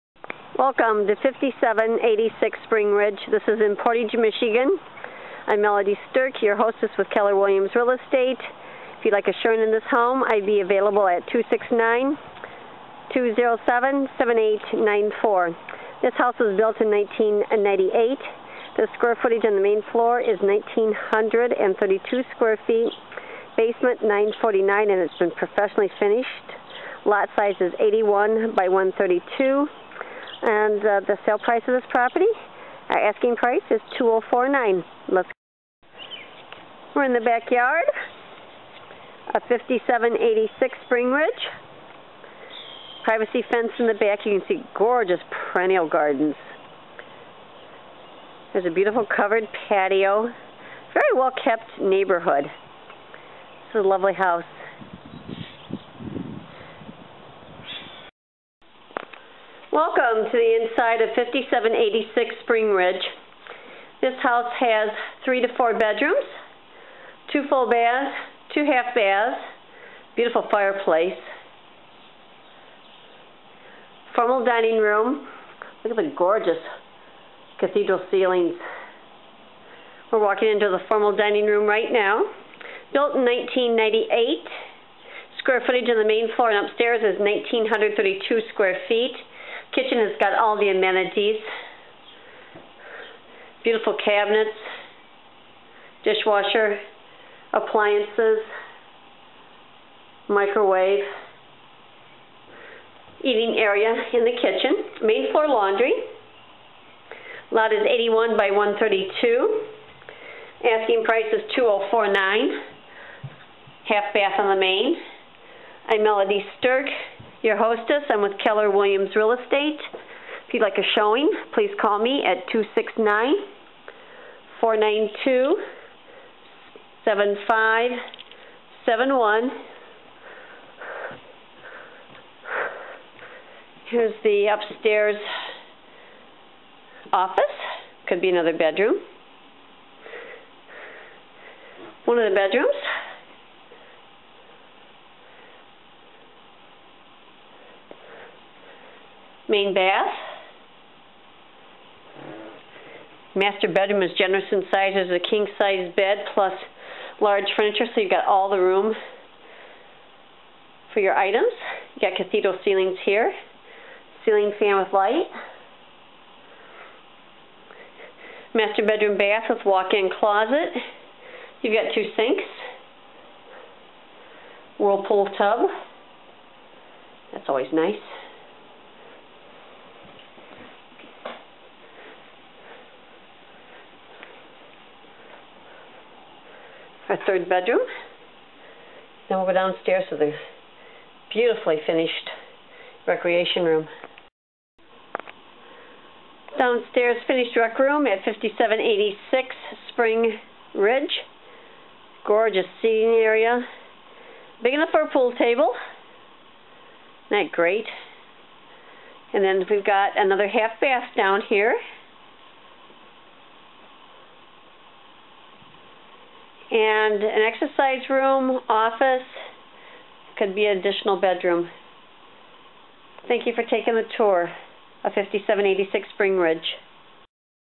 5786 Springridge
Portage Schools! As you enter this great traditional style home you will notice the warm living room offers cathedral ceilings, gas log fireplace leading into the formal dining room. THe kitchen has oak cabinets, gas range, nice deep sink, quartz countertops, pantry, garbage disposal, dishwasher, and eating area which has sliders to the back yard, covered patio and hot tub. There is main floor laundry and a 1/2 bath rounding out this level. Upstairs you will notice the open office area, nice size master bedroom with cathedral ceilings, master bath offers 2 sinks, oak cabinets, shower and 6 Ft Jacuzzi tub and walk in closet, the other 2 bedrooms upstairs are a nice size. THe basement has a rec room big enough for a pool table, 1/2 bath, 4th bedroom or exercise room, the utility room has a water heater on 3 years old, Bryant furnace and Central Air. Great Yard with inground sprinklers. This impeccable house is just what your looking for, come take a look.